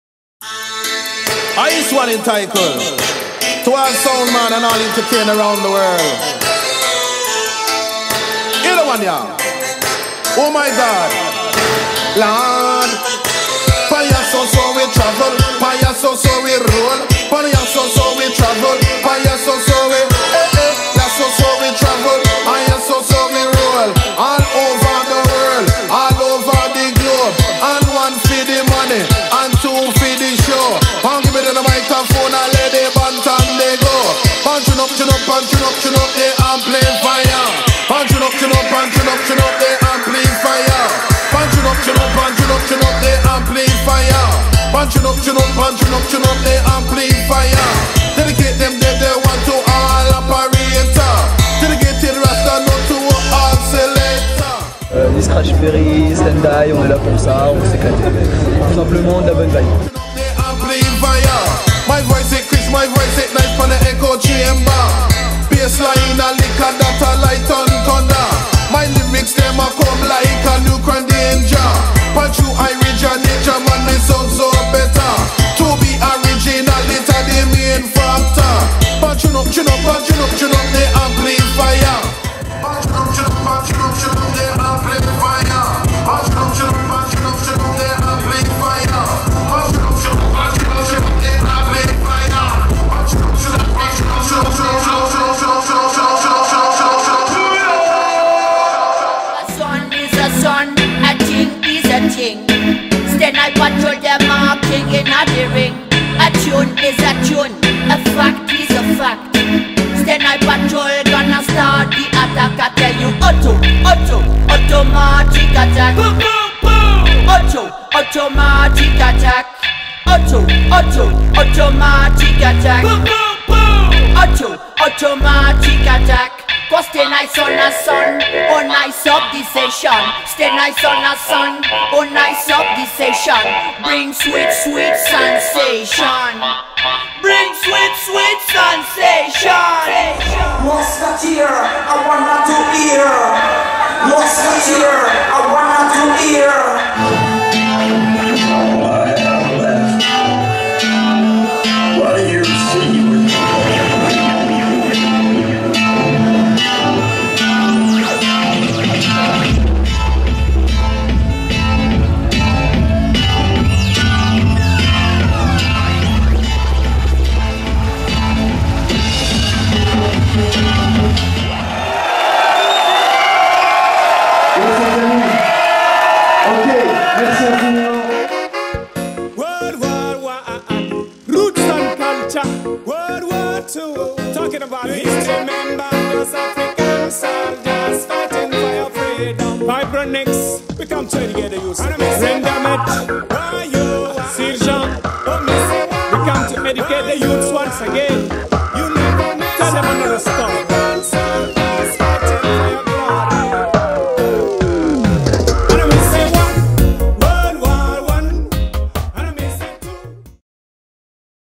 Télérama Dub Festival vol.11 - Extraits Live - © Culture Dub
Voici quelques images du Télérama Dub Festival vol.11 qui a notamment eu lieu à Paris au 104 les 22 et 23 Novembre 2013. Une bien belle édition avec la crème des dubbers du monde entier : Iration Steppas, Stand High Patrol, Brain Damage, Vibronics, Dub Colossus, Dubakill, Adrian Sherwood, RSD, Panda Dub...

Merci à Télérama, Fred, Bi-Pole, Cyril, Olivier, et tous les artistes présents !